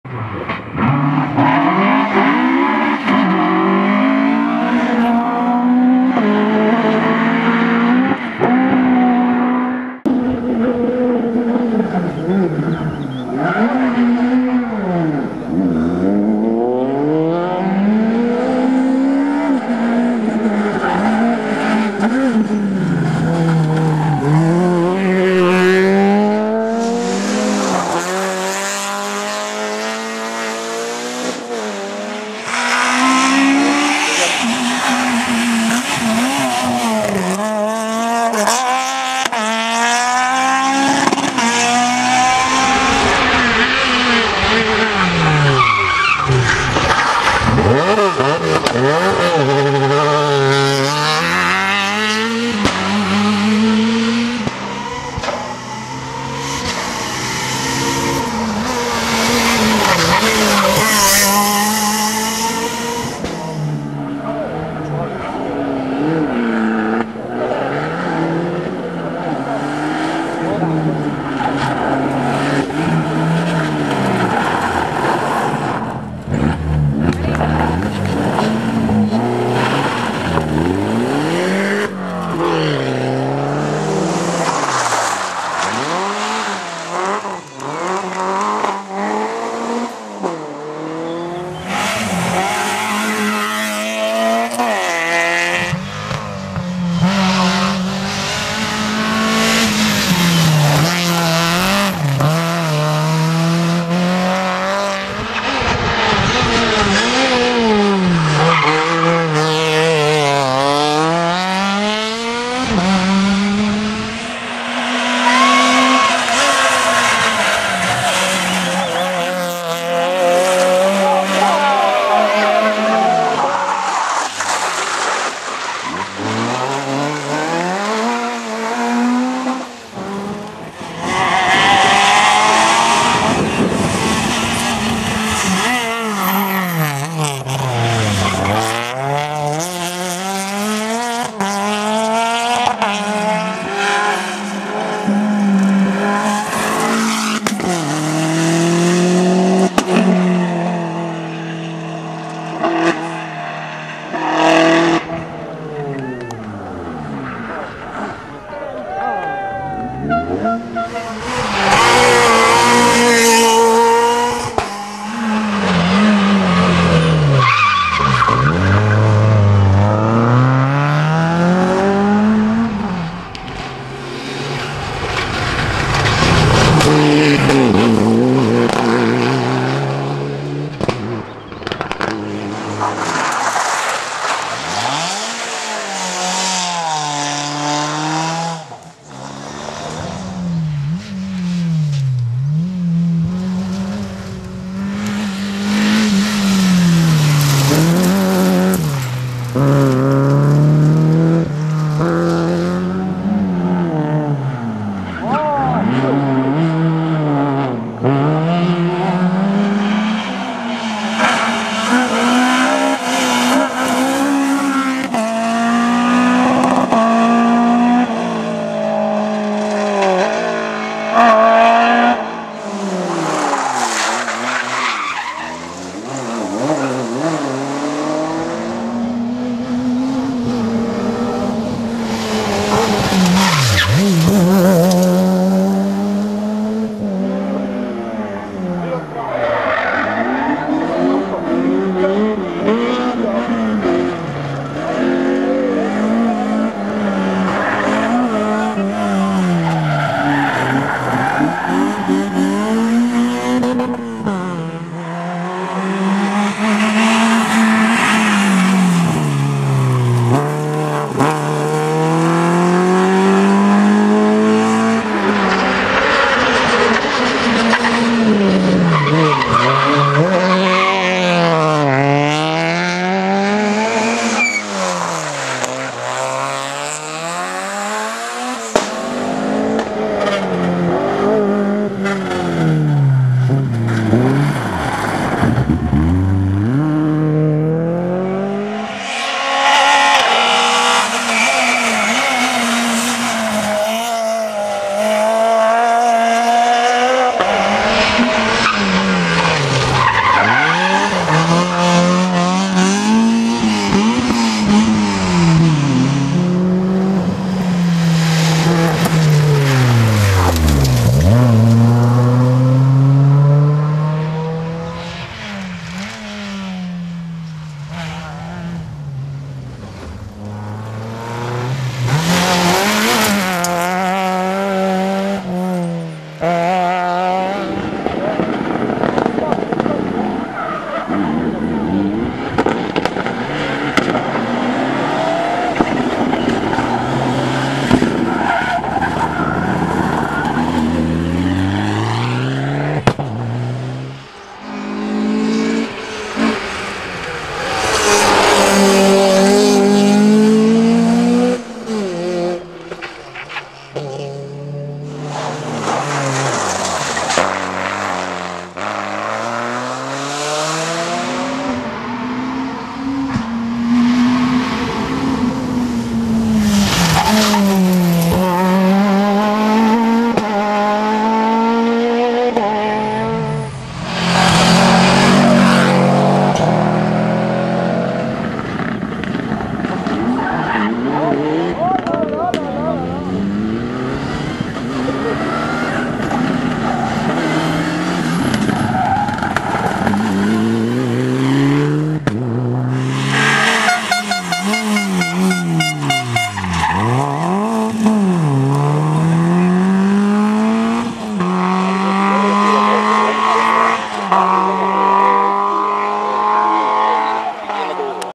Rallye du Val Dadou 2015
Voici la bande annonce de notre DVD du rallye du Val Dadou 2015 comprenant environ 15 passages de tous les équipages...Vous pouvez commander votre DVD sur notre site internet...